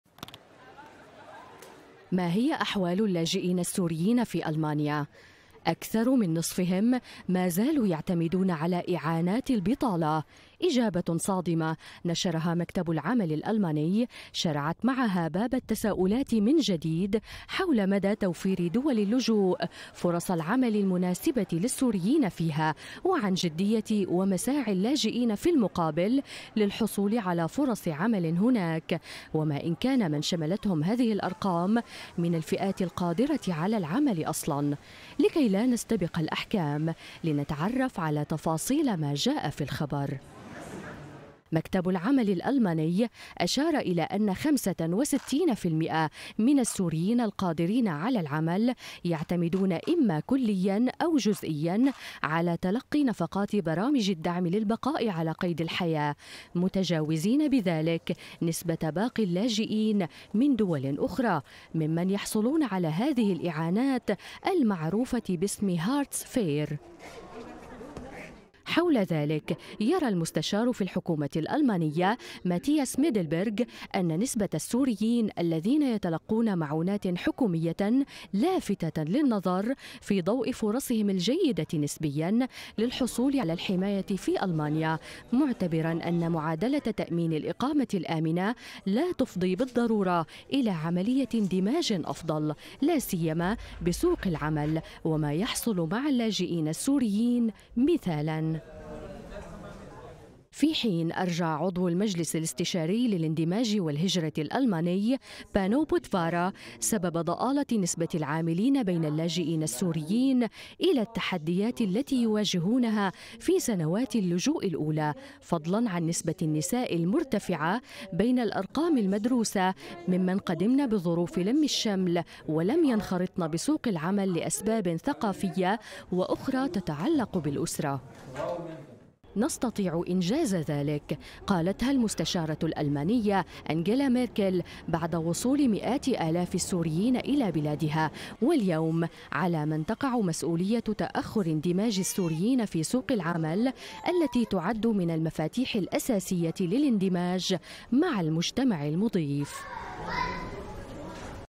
أرقام صادمة حول البطالة بين السوريين في ألمانيا ما أسبابها؟
أعلنت الوكالةُ الاتحادية للتشغيل في ألمانيا أنّ ما يقربُ من ثلثي السوريين القادرين على العمل في ألمانيا، يعتمدون كلياً أو جزئياً على إعانات البطالةِ المعروفة باسمِ "هارتس فير"، وأظهرت إحصائيةٌ جديدة للوكالة أن نسبةَ متلقي الدعمِ الحكومي بين السوريين بلغت خمسةً وستينَ بالمائة في آذار الماضي، وهي تفوقُ بكثيرٍ نسبَ باقي اللاجئين من دول أخرى
إعداد: مريم محمد
قراءة: أحلام إبراهيم

هوتبيرد HD
V 12520